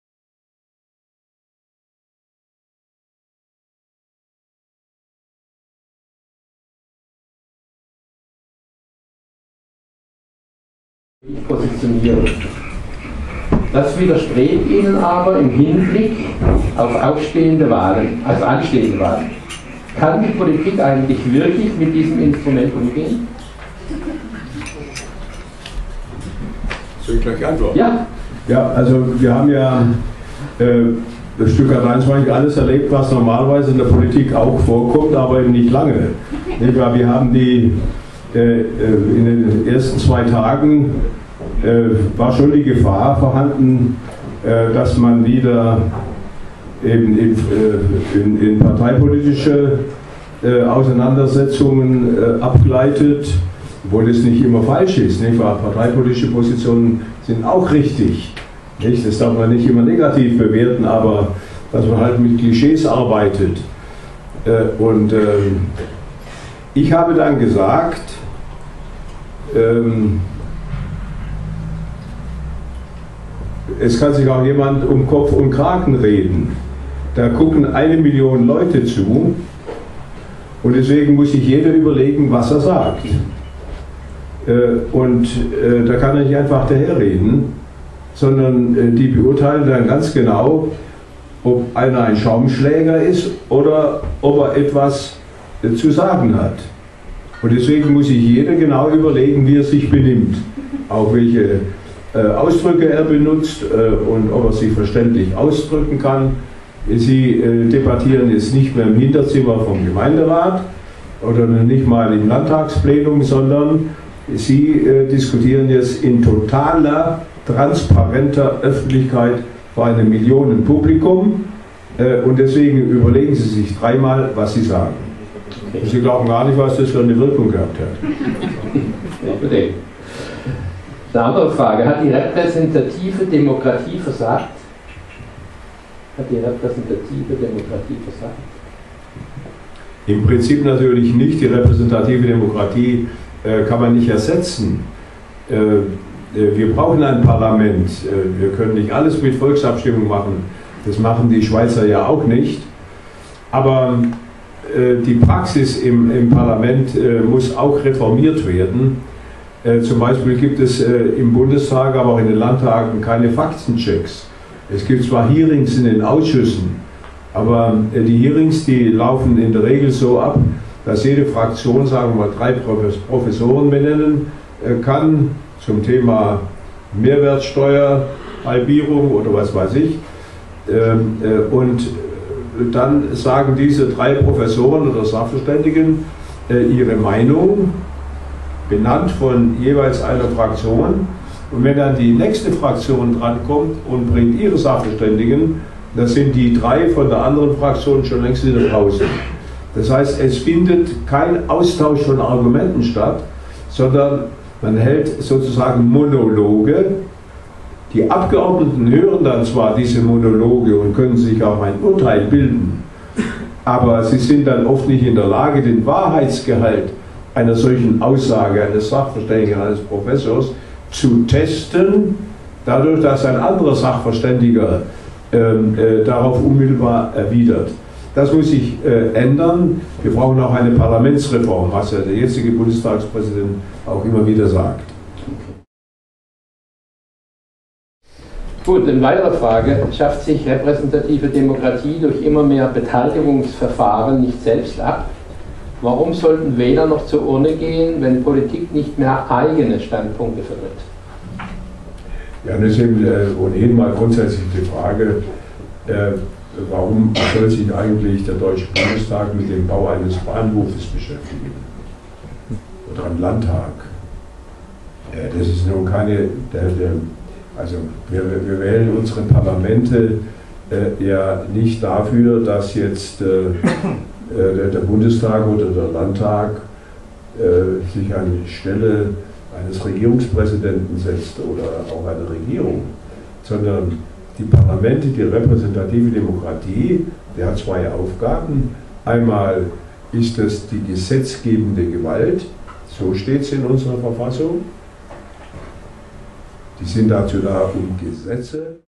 Dr. Heiner Geißler im Gepräch mit Rolf Birmelin
Am 22. November 2011 traf sich Dr. Heiner Geißler (Bundesminister a.D.) auf der EVENTbühne in Kimratshofen mit Rolf Birmelin um das Thema "Mehr direkte Demokratie in Politik und Unternehmen" zu besprechen.